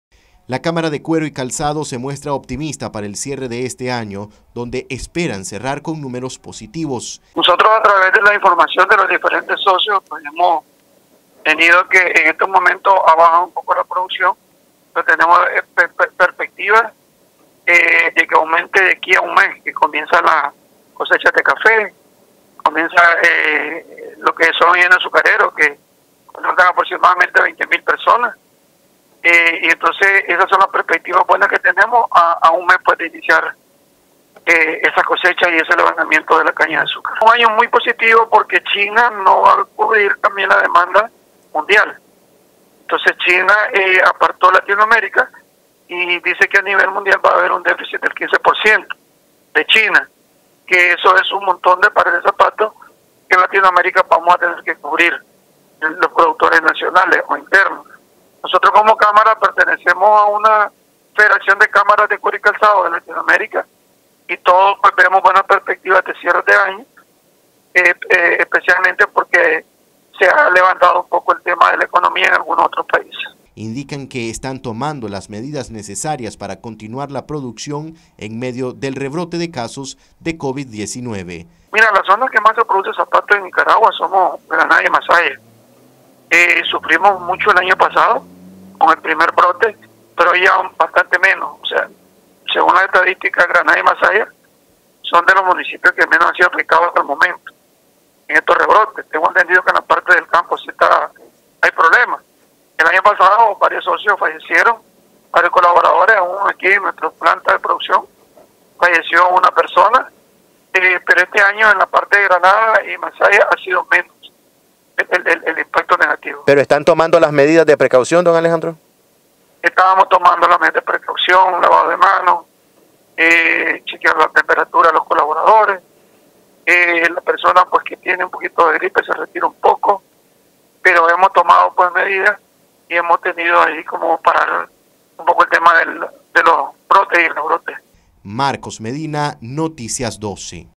Sector Cuero Calzado con altas expectativas para el cierre de año
Noticias12Nicaragua Sector Cuero Calzado con altas expectativas para el cierre de año.